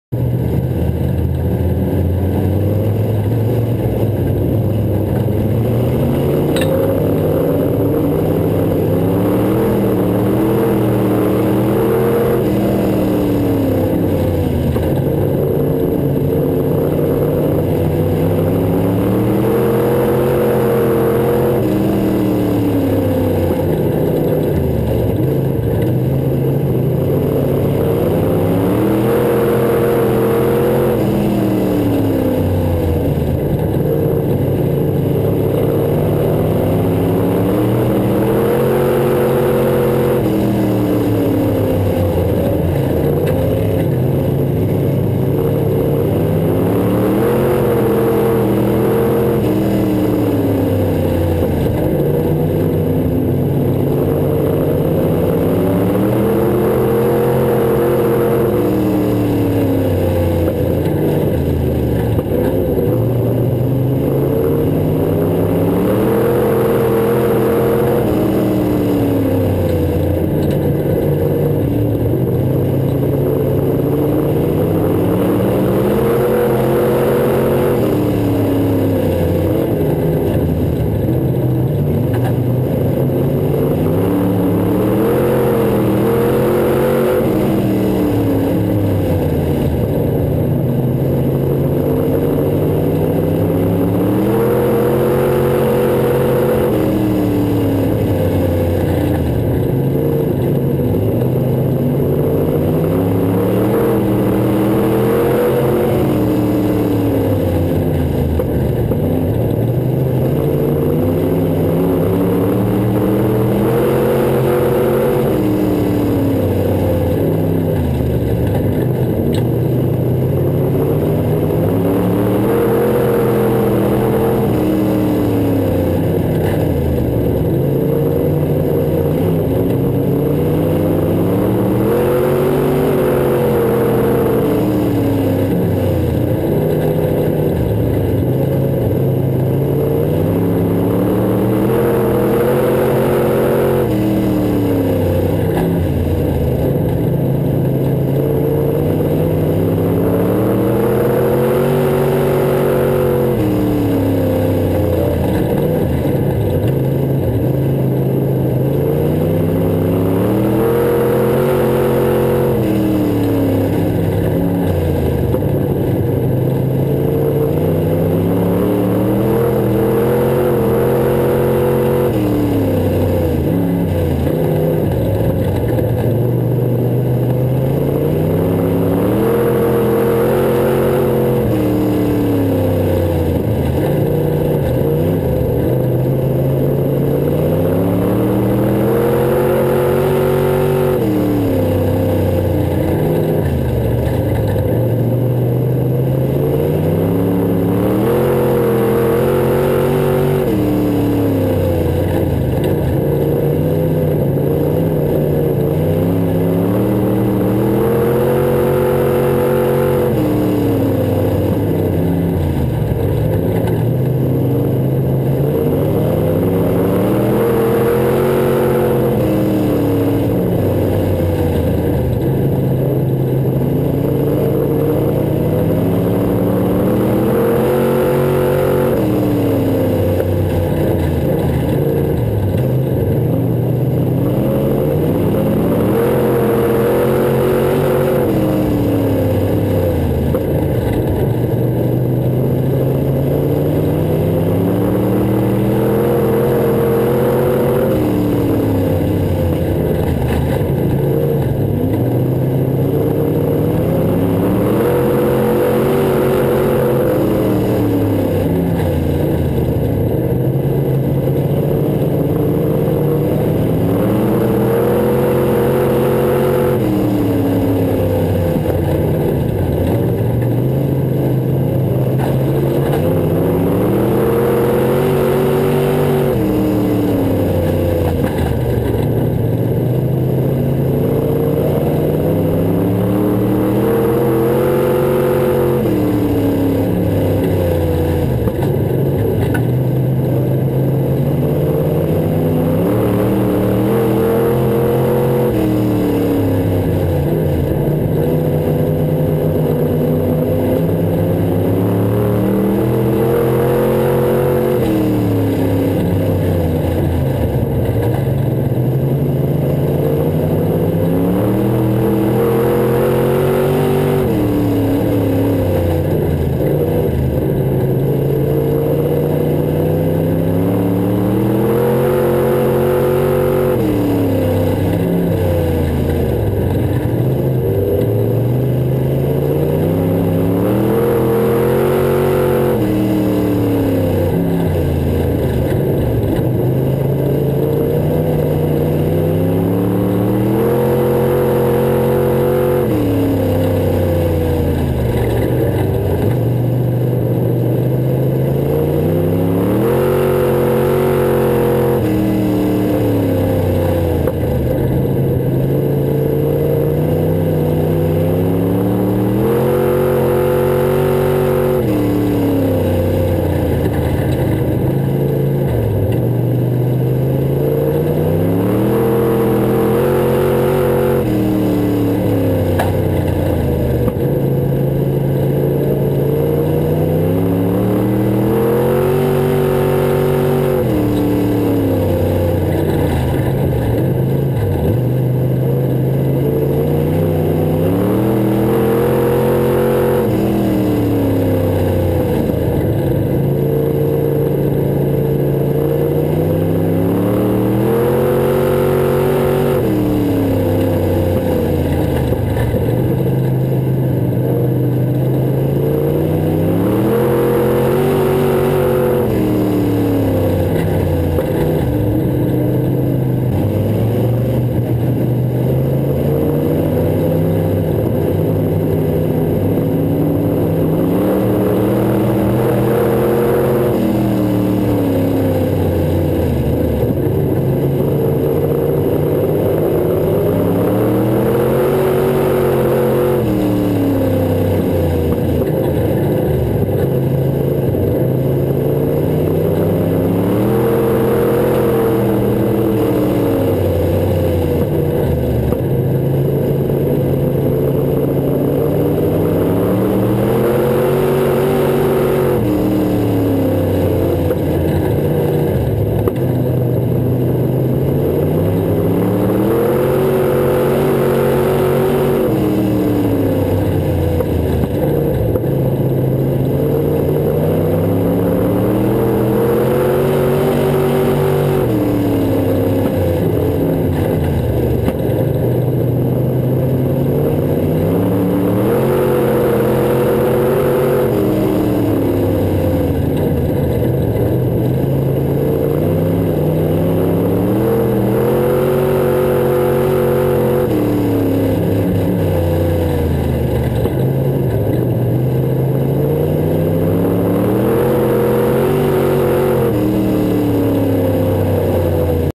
imcastyle Chris Budzban IMCA Northern Sportmod Feature Luxemburg Speedway 9/15/12
Feature action at Luxemburg Speedway on 9/15/12. Started 5th and fell back to 14th when the caution flag flew. Got a good run at the end to climb back to 11th at the checkers. Remember to check us out on the web